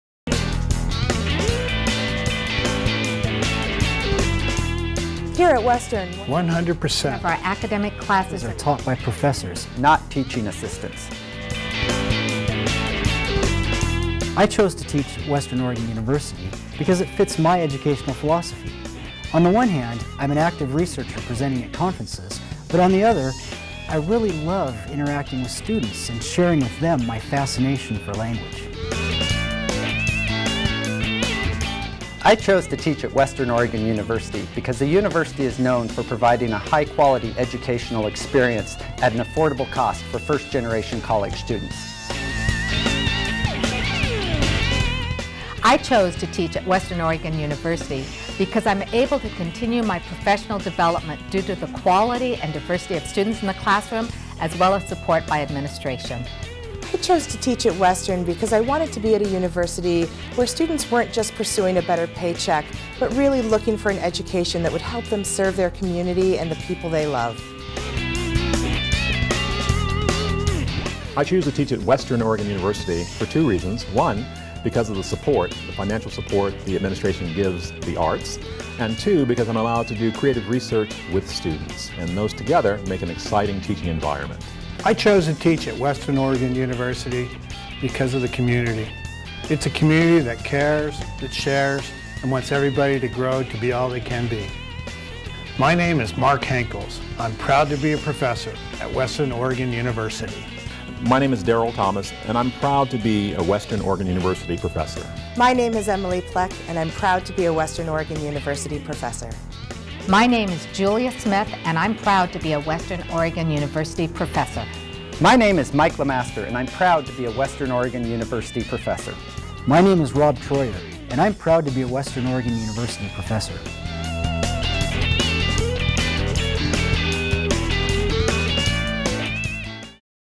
Faculty Choose WOU
Western Oregon University faculty members explain why they teach at WOU.